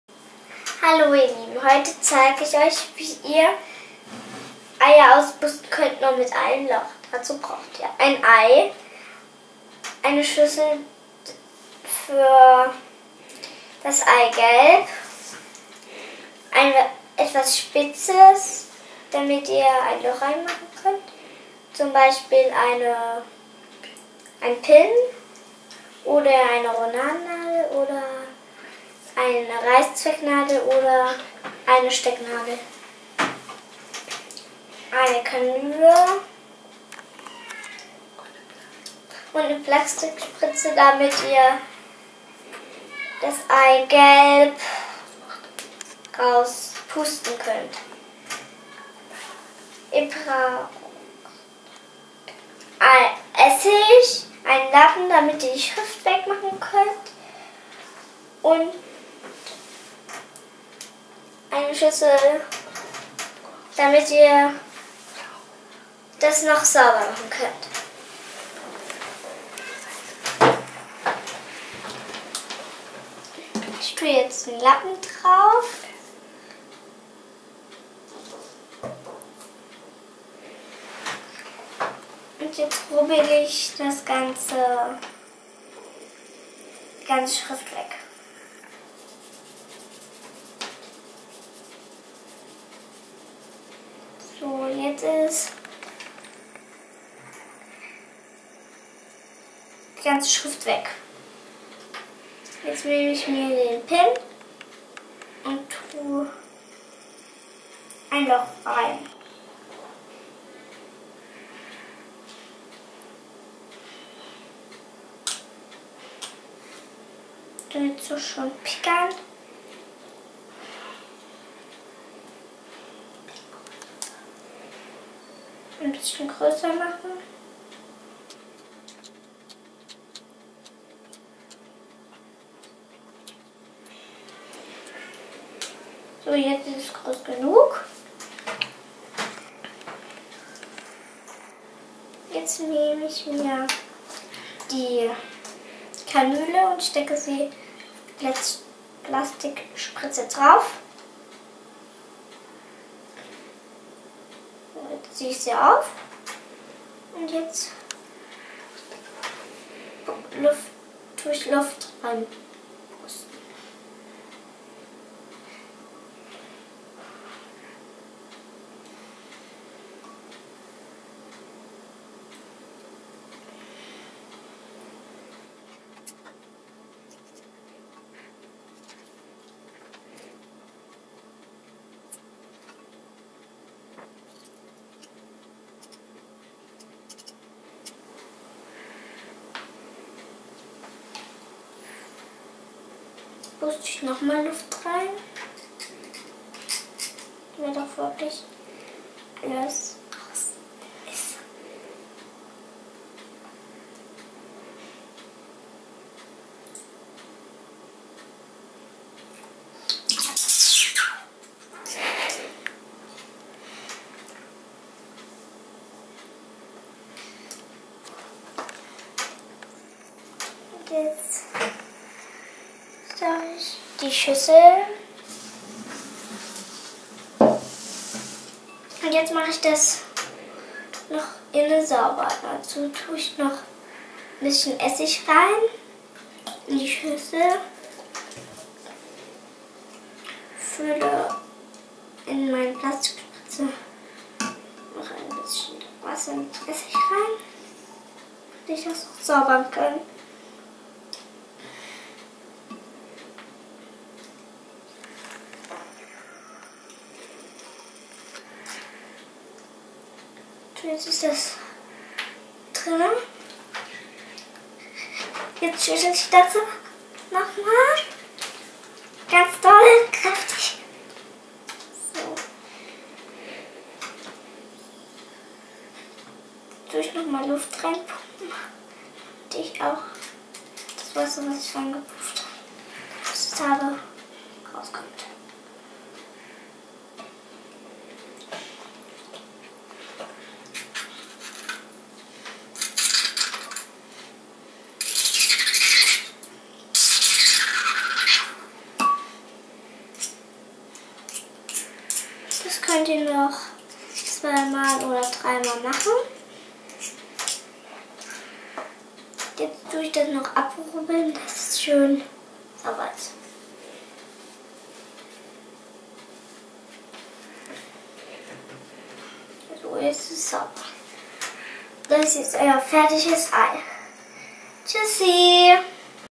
Eier mit nur einem Loch auspusten - mit Spritze und Kanüle
In diesem Video zeigt euch Laura, wie ihr eure Eier, die ihr später bemalen könnt, mit nur einem Loch auspustet. So sehen sie nach der Bemalung schöner aus. Dazu braucht ihr: zwei Schalen, ein Tuch, Essig, ein spitzer Gegenstand (z.B. eine Reißzwecke, eine Stecknadel oder eine Roladennadel) eine Plastikspritze und eine Kanüle genügen.

0:00 Was ihr braucht
1:11 Ei säubern
1:43 Ei auspusten
3:48 Ei innen saubermachen

Wenn euch unsere Videos gefallen, dann aboniert doch gerne unseren Kanal.